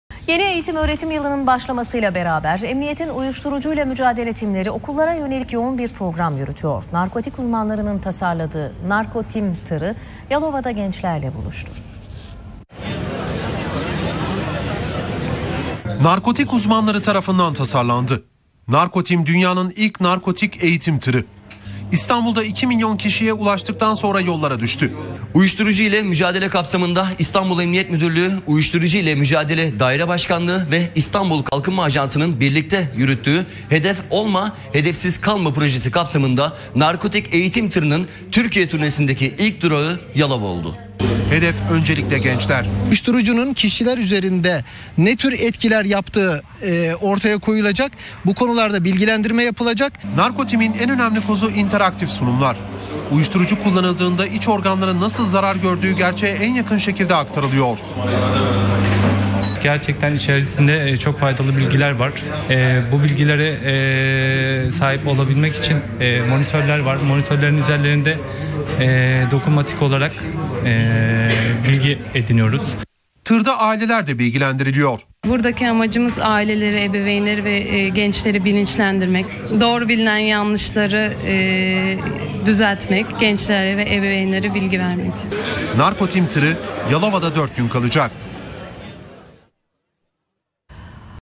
Narkotim Tırı Yalova'da
Uyuşturucu ile mücadele kapsamında hayata geçirilen Narkotim Tır'ı Türkiye turnesine Yalova ile başladı.